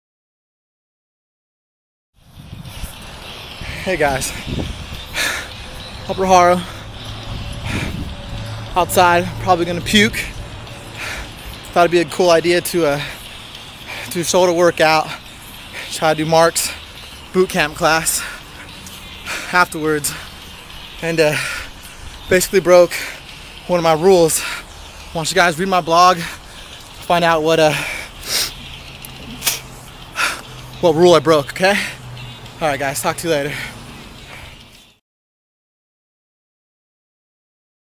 Breaking The Rules The Next Level Gym Call Today 909-465-07
Owner of Chinos personal training gym demostrates that you shouldnt break fitness rules.